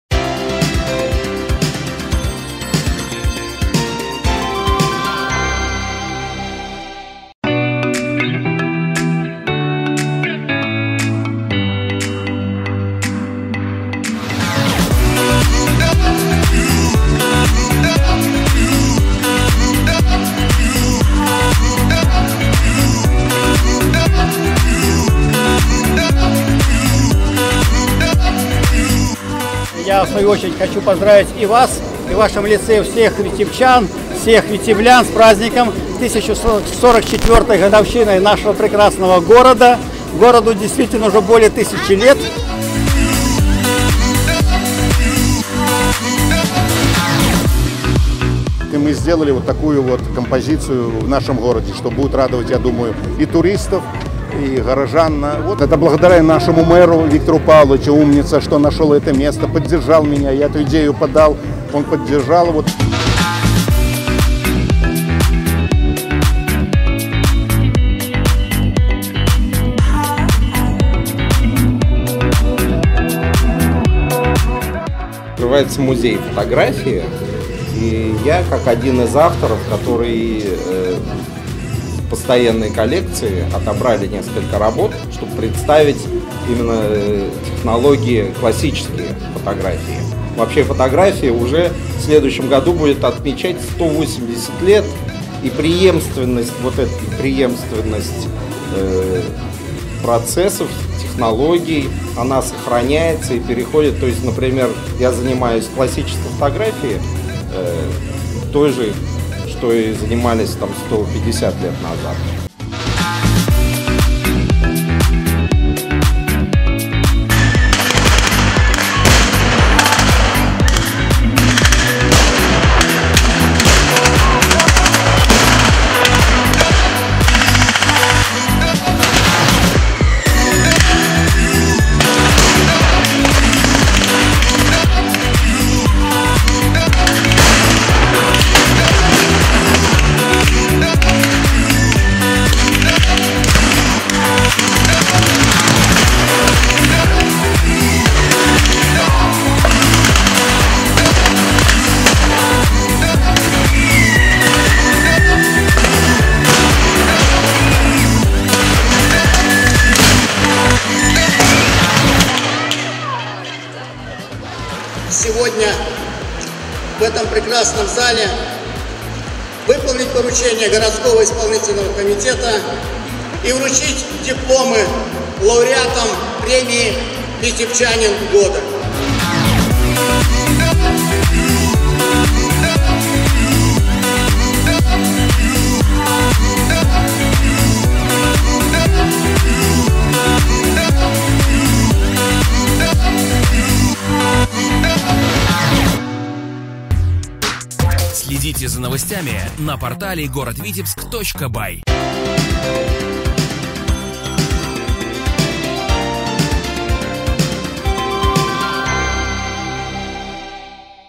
День города Витебска 2018. Как отметили день города Витебска?
Витебчане с размахом отпраздновали 1044-ю годовщину города над
Двиной. В День города для жителей и гостей Витебска были запланированы
различные мероприятия. На улицах и площадях, в парках и скверах прошли
десятки мероприятий: открылись новые скульптурные композиции,
своим творчеством порадовали различные коллективы, проводились
выставки, акции, презентации, квесты и мастер-классы.